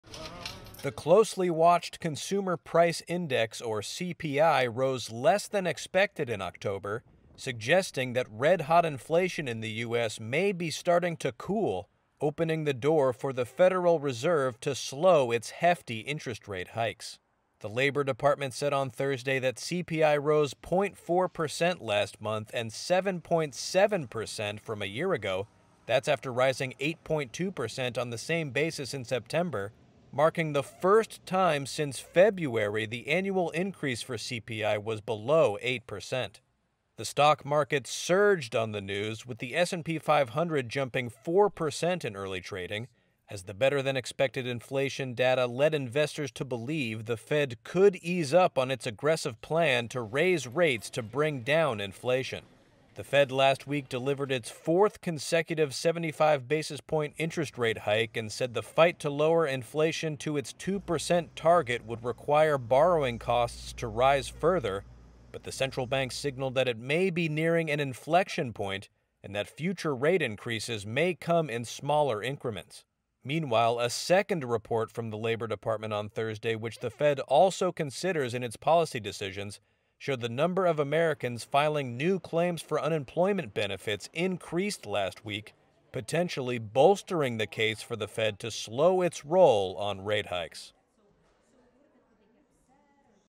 U.S. inflation eases more than expected: report
U.S. consumer prices rose less than expected in October, pushing the annual increase below 8%, the strongest signs yet that inflation was slowing, which would allow the Federal Reserve to scale back its hefty interest rate hikes.